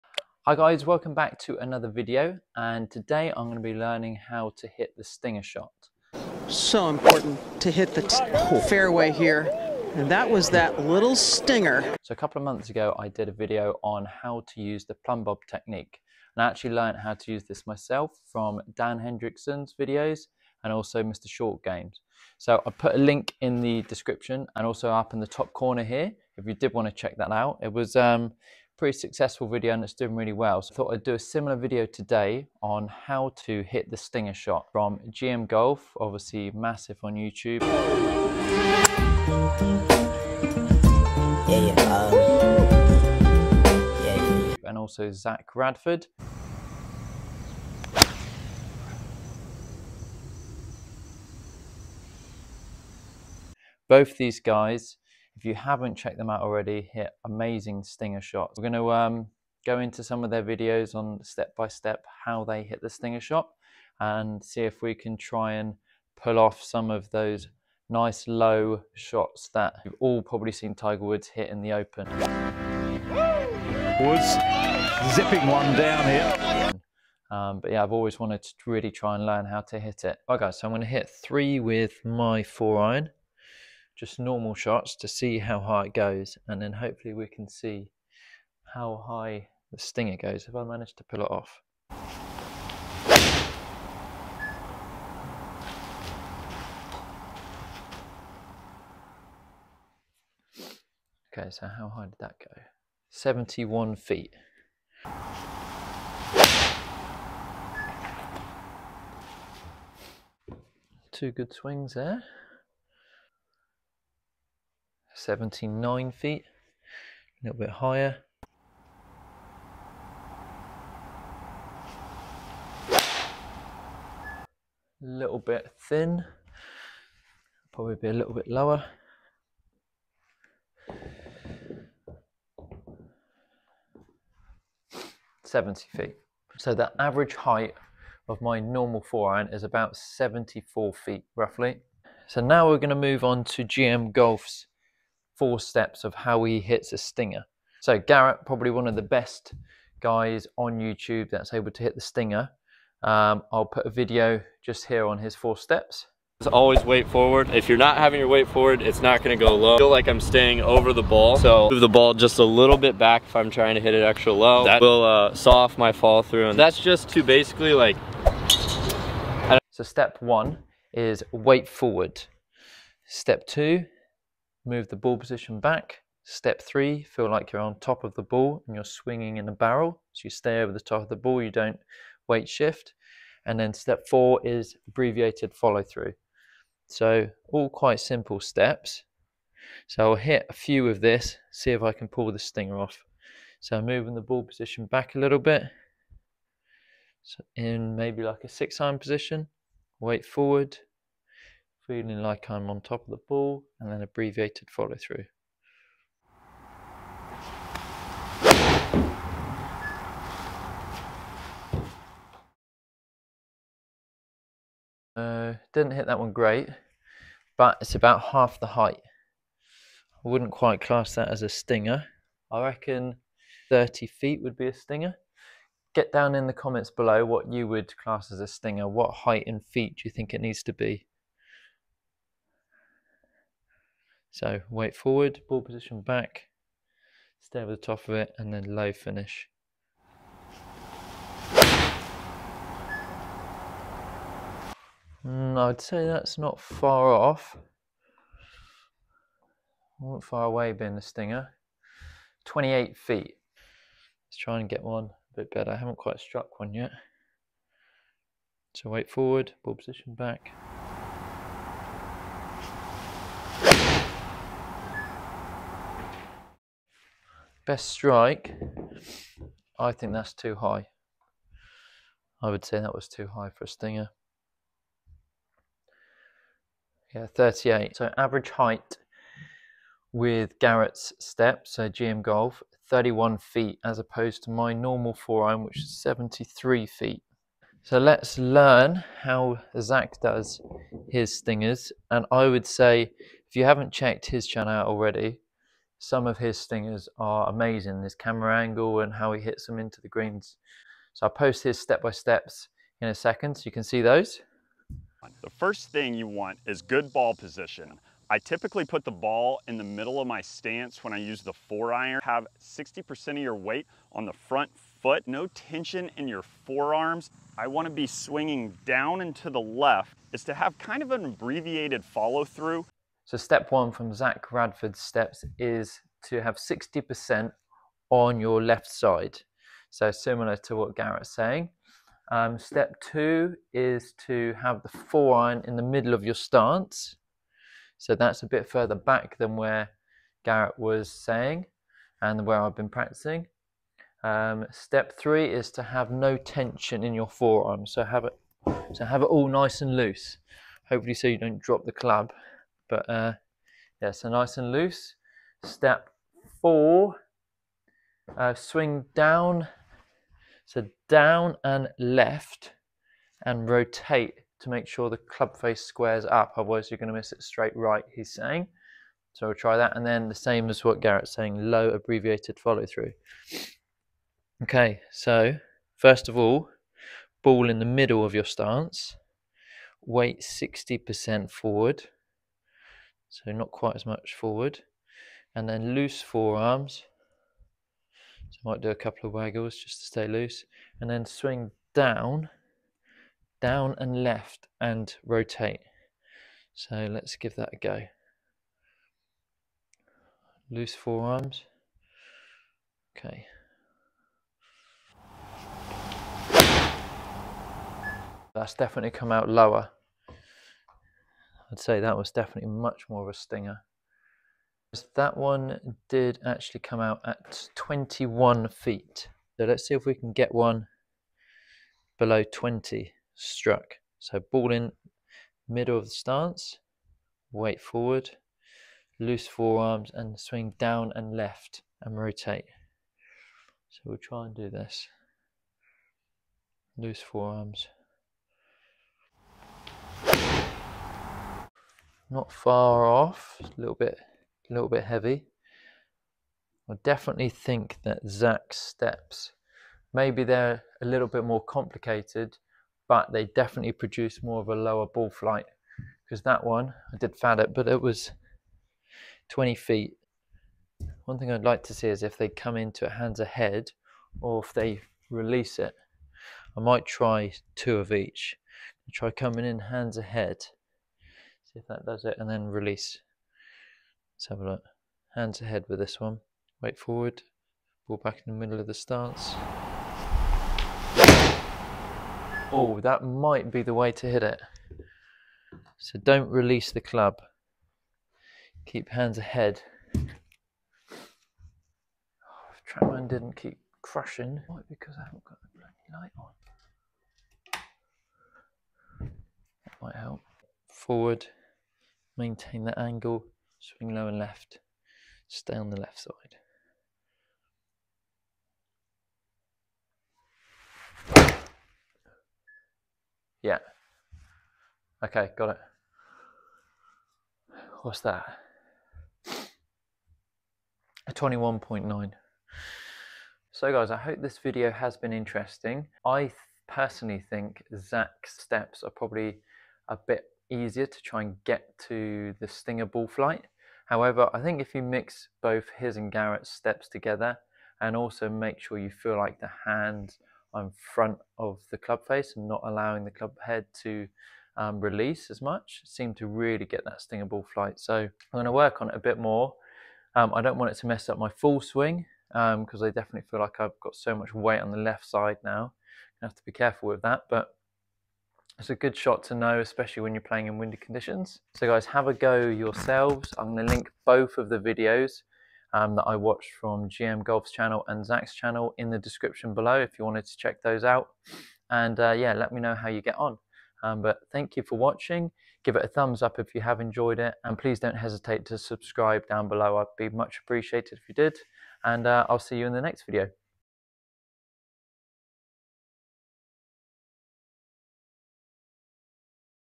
How to Hit The PERFECT Stinger Shot! w/ Help from GM Golf & Zac Radford
Learn how to perfect the stinger shot with this comprehensive step-by-step guide. I will try out tips from GM Golf (Garrett Clarke) and Zac Radford on the basics of setting up, understanding the angle of the clubface, making adjustments to the clubhead, and then executing the stinger with power and accuracy. This tutorial will help you master this shot, so you can start hitting the fairway and score lower on the golf course!

 @gmgolf   @zacradford   @Alex Whittle Golf

Clips used from @gmgolf @ZacRadford @PGATOUR @TheOpen all used for educational examples.

Also I have various artwork for sale too for anyone that is interested. 👇